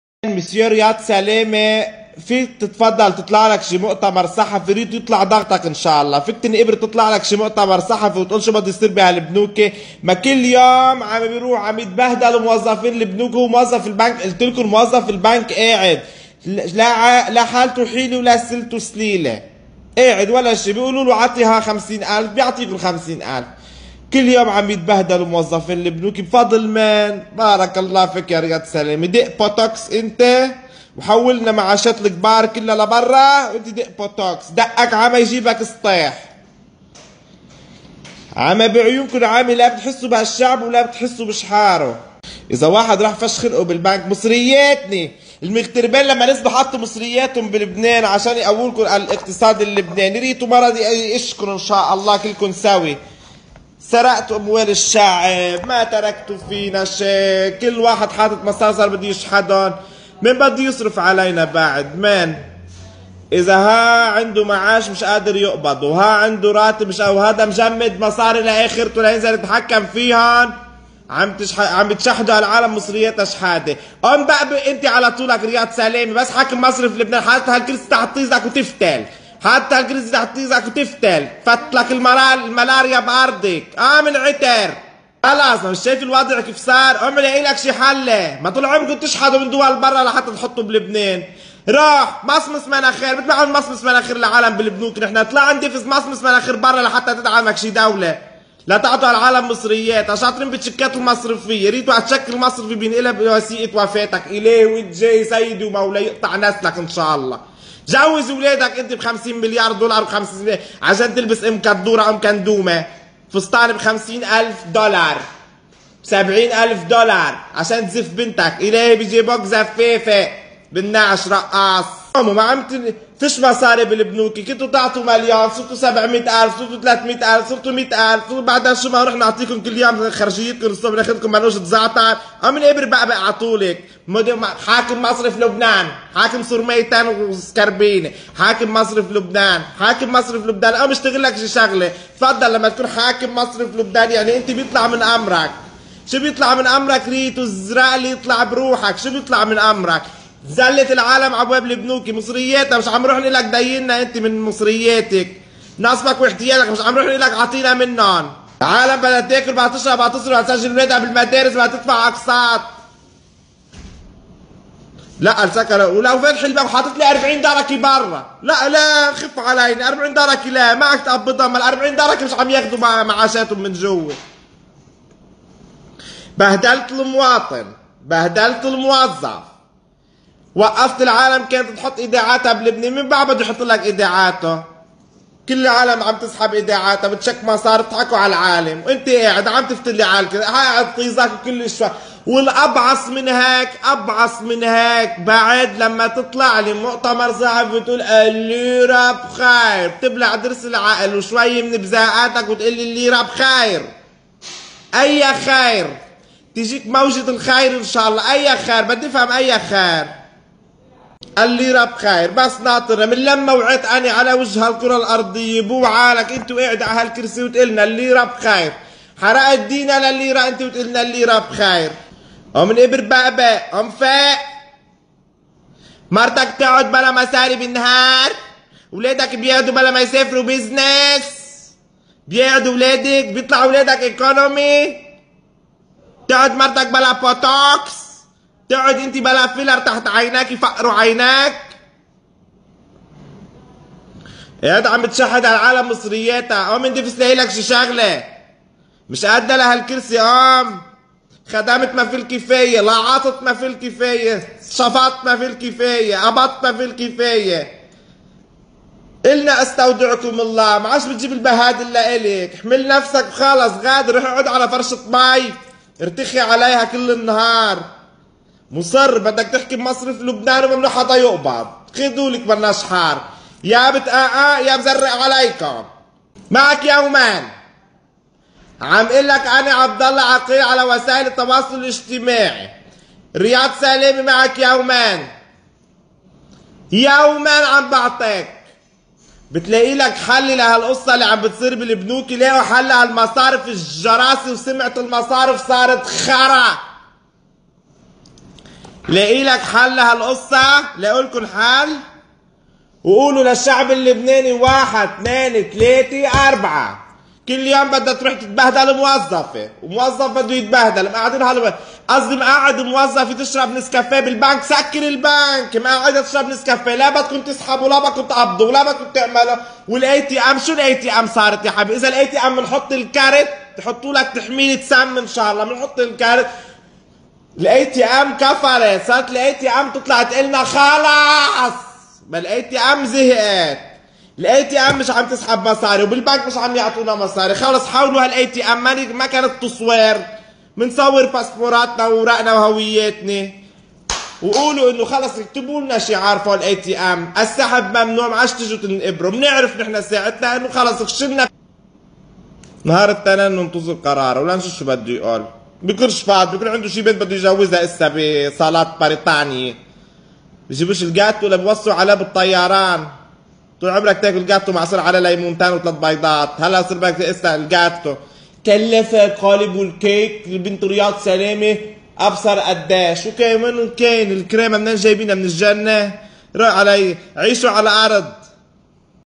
انتقاد رياض سلامة - Criticizing Riad Salameh
كرمال هالفساد و الاهمال و التقصير واستخفاف بالشعب  ...
 وكرمال هدر مال العام والسرقة  ...
والمحسوبيات والرشوات .
 كرمال كل شي بشع نزلنا ...
عم تسئلوا لي نزلنا... نزلنا كرمال الانسان وكرمال لبنان...
 ثورة ١٧ تشرين١